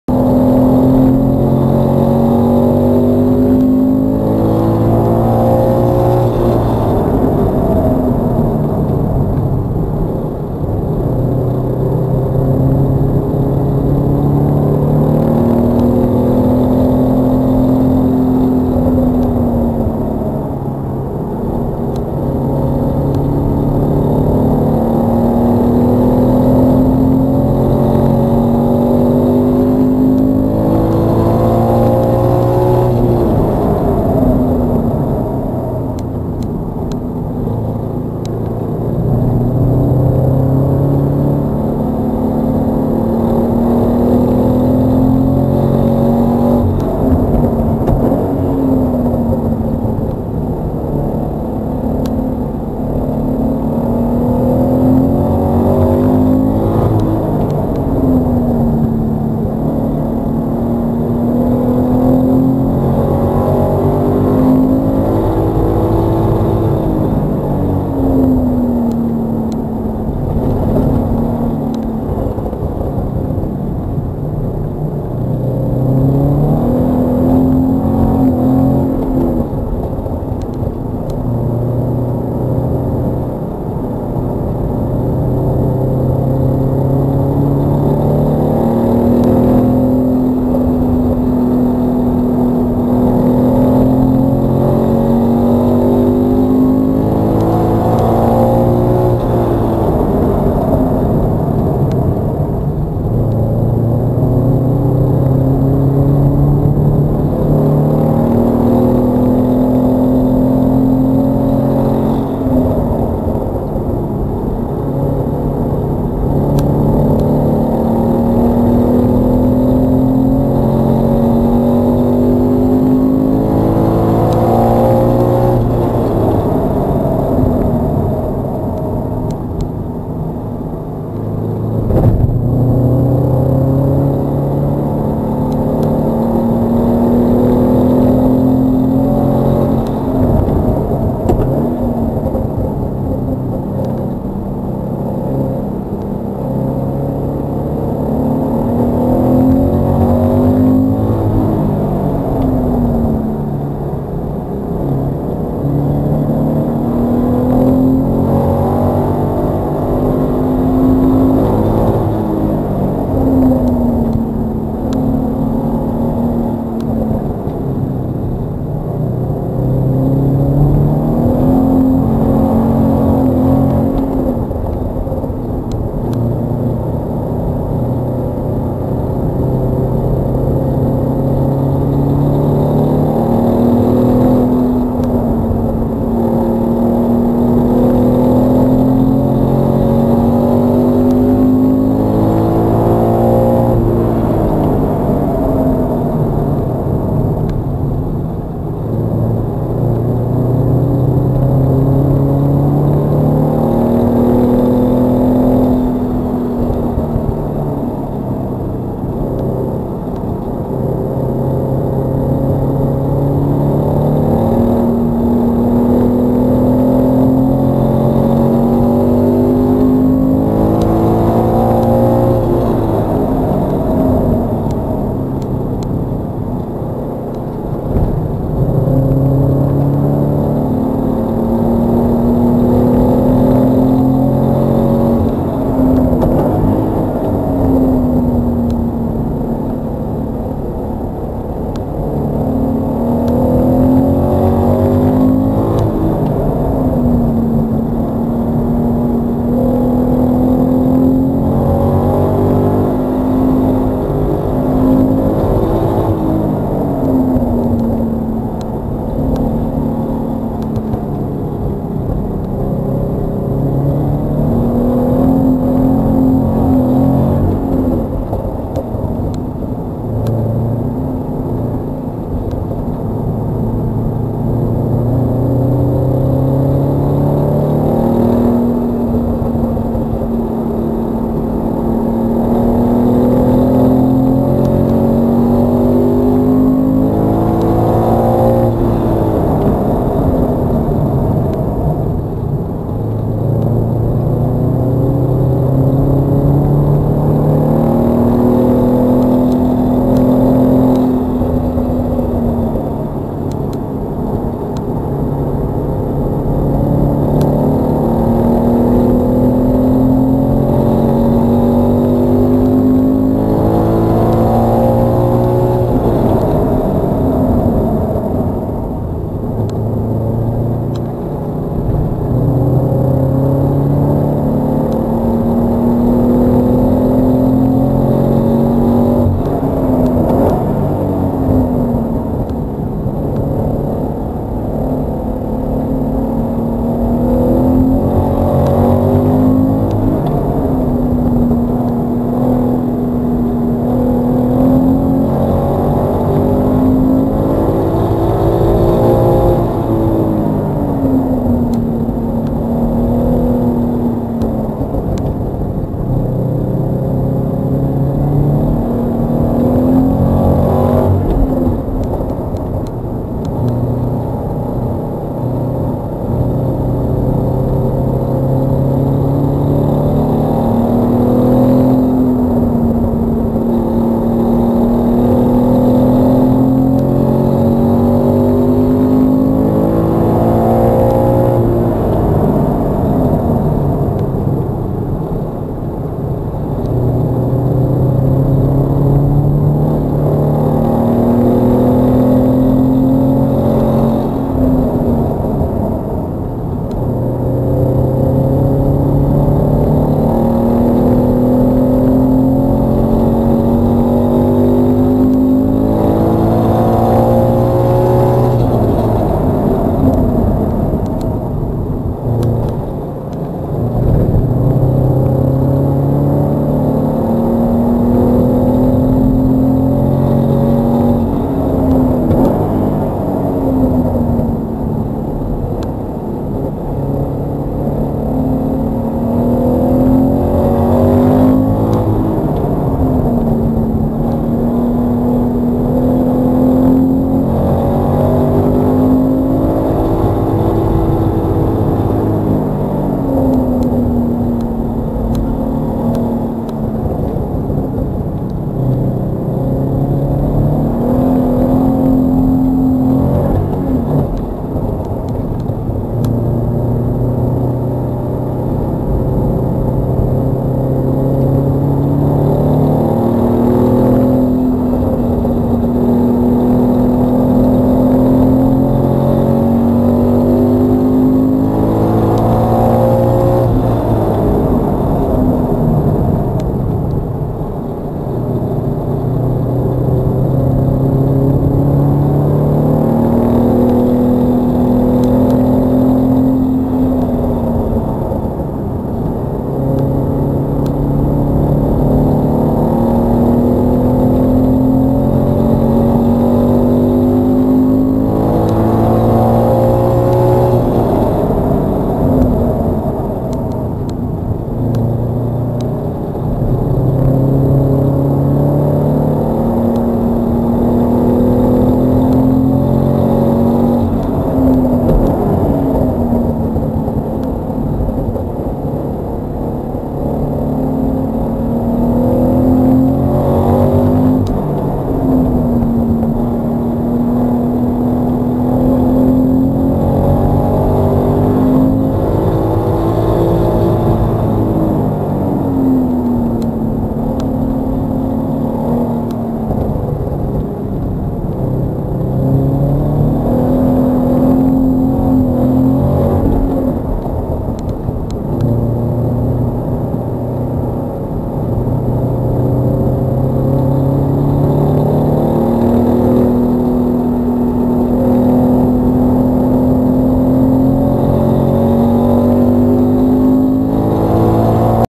Meppen wet laps (I finally got new wheels!)
After a long break, I am back with a new video.

Unrelated to my incident on they NBR last year (as might be the impression since I stopped uploading videos) I had a slump in motivation for driving for other personal reasons. I did, in fact, drive a couple of laps on the NBR again last year on stock wheels with 5 years old Michelin Primacy tires. And even worse, I had changed to stock brake pads as well, and they started to fade just a few minutes into each lap. So the videos were very uninteresting to watch and I didn't bother to upload them.

However, I didn't want to miss the yearly Subura Drivers Club meeting at Racepark Meppen, so I decided to get a set of Protrack One wheels. Everything was done at the last moment and I didn't get to replace a broken camera mount. So I slapped the camera onto my helmet to at least get some video. The quality is less than stellar, but watchable nevertheless...